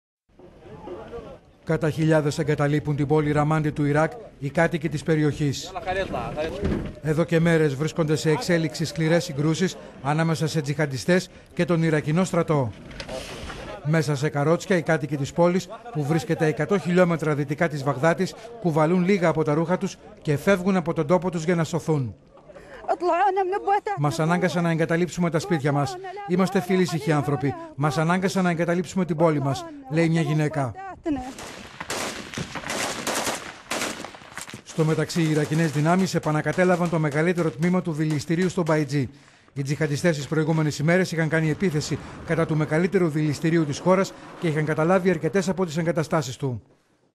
Ιράκ: Χιλιάδες ξεριζωμένοι από την πόλη Ραμάντι λόγω τζιχαντιστών
Κατά χιλιάδες εγκαταλείπουν την πόλη Ραμάντι του Ιράκ οι κάτοικοι της περιοχής.

Εδώ και μέρες βρίσκονται σε εξέλιξη σκληρές συγκρούσεις ανάμεσα σε τζιχαντιστές και τον ιρακινό στρατό.

Μέσα σε καρότσια οι κάτοικοι της πόλης, που βρίσκεται 100 χλμ. δυτικά της Βαγδάτης κουβαλούν λίγα από τα ρούχα τους και φεύγουν από τον τόπο τους για να σωθούν.

Στο μεταξύ, οι ιρακινές δυνάμεις επανακατέλαβαν το μεγαλύτερο τμήμα του διυλιστήριου στο Μπαϊτζί.